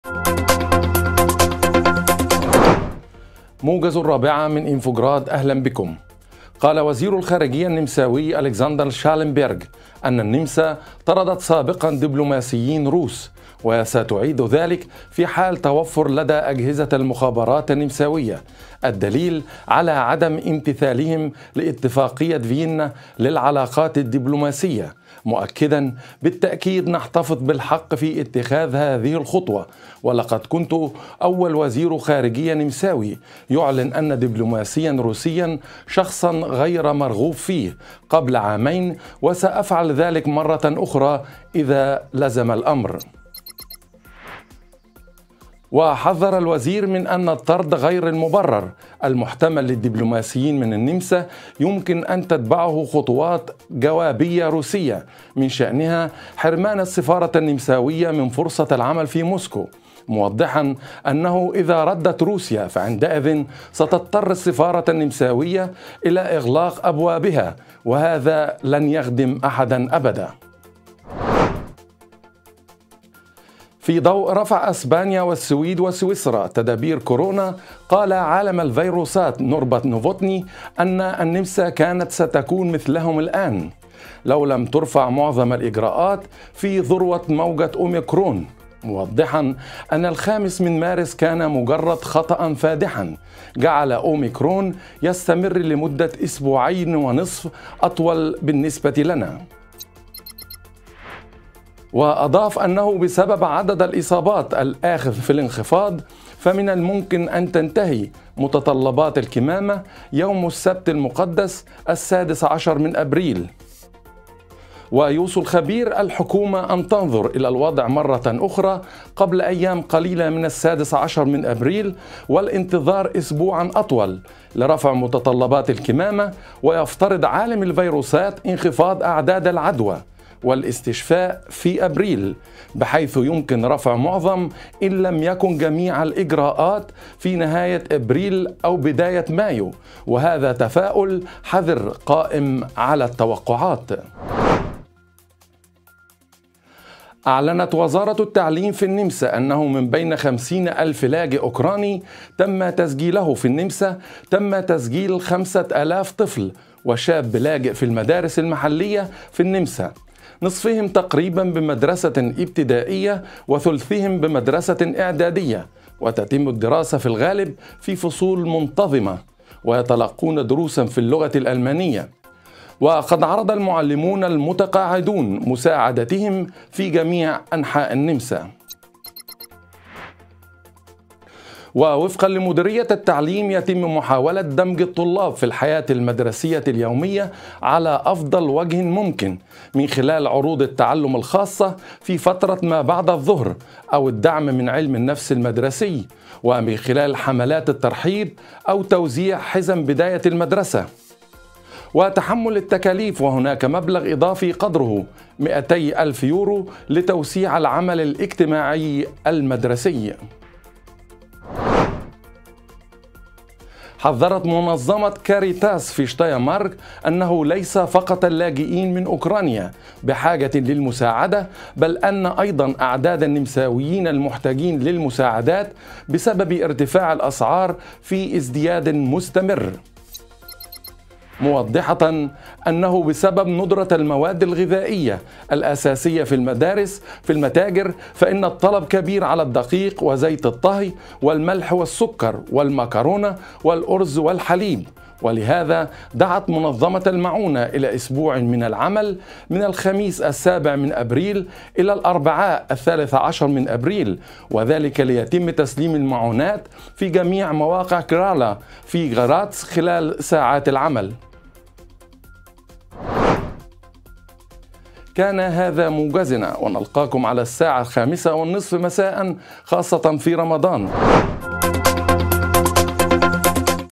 موجز الرابعة: النمسا لا تستبعد طرد السفير الروسي وارتفاع مستوى الفقر في البلاد
النمسا لا تستبعد طرد السفير الروسي وارتفاع مستوى الفقر في البلاد
موجز الرابعة لأنباء النمسا من INFOGRAT ليوم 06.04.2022